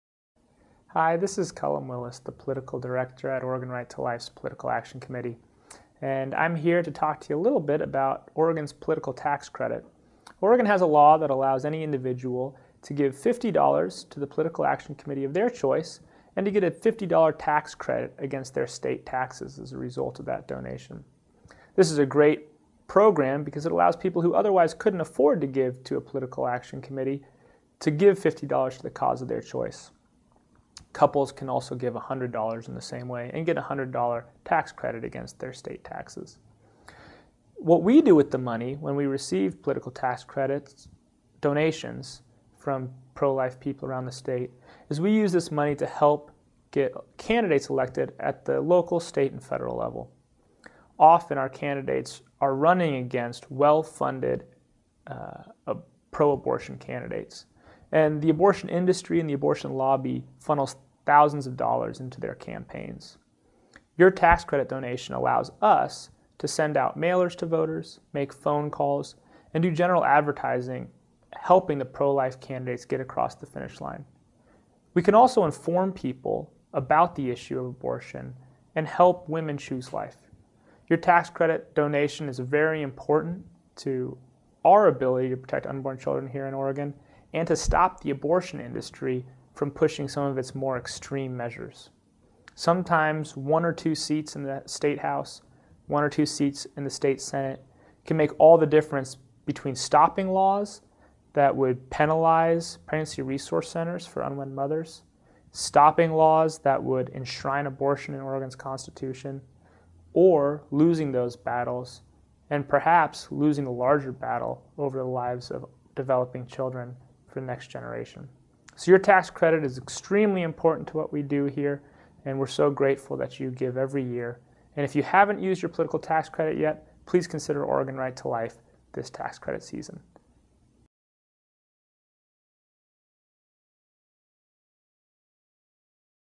Don't Forget About Your Political Tax Credit!
Your political tax credit donation is vital in helping to support pro-life candidates who will defeat pro-abortion legislation here in Oregon. If you haven't already, please consider giving your $50 political tax credit to Oregon Right to Life and help us fight to protect women and their babies.